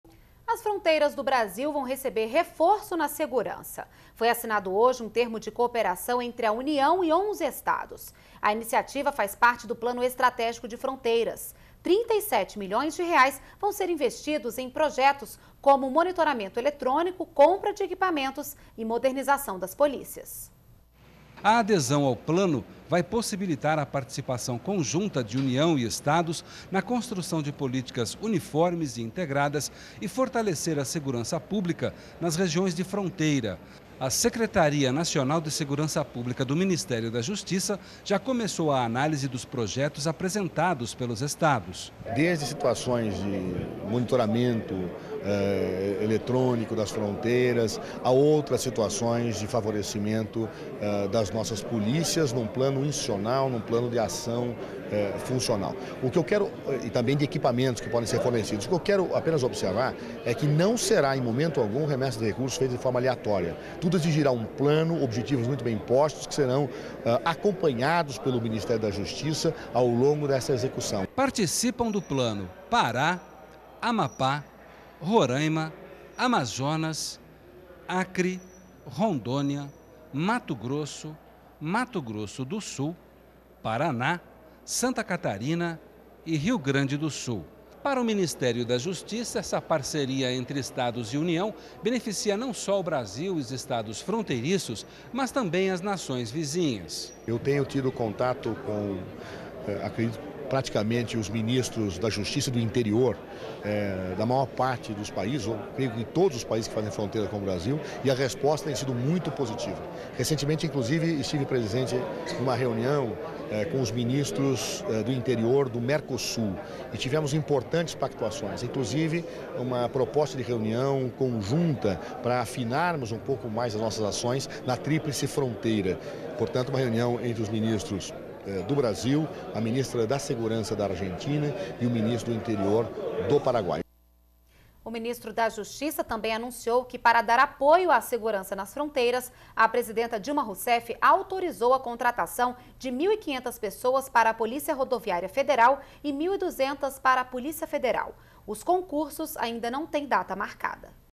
Parceira entre União e Estados vai reforçar segurança nas fronteiras do país
NBR Notícias - 08.12.11: As fronteiras do Brasil vão receber reforço na segurança. Foi assinado nesta quinta-feira (08), um termo de cooperação entre a União e 11 estados. A iniciativa faz parte do Plano Estratégico de Fronteiras. Trinta e sete milhões de reais vão ser investidos em projetos como monitoramento eletrônico, compra de equipamentos e modernização das polícias. O ministro da Justiça, José Eduardo Cardozo, também anunciou que para dar apoio à segurança nas fronteiras, a presidenta Dilma Rousseff autorizou a contratação de 1.500 pessoas para a Polícia Rodoviária Federal e 1.200 para a Polícia Federal. Os concursos ainda não tem data marcada.